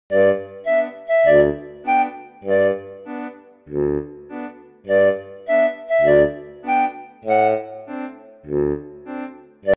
つくしの卵とじ(隠し味にツナmix）(*´з`)
今年も～

料理得意な奥さんに甘えてみましたｗ

マジで美味い( *´艸｀)

この時期以外はレア食材？

普段　こやつは中々　食べられません

つくし捕獲中にポケwifi一式紛失して

高くつきましたｗｗｗ

5年愛用してたAterm MR05LN＆SIMちゃん　さようなら～

(´ﾟдﾟ｀)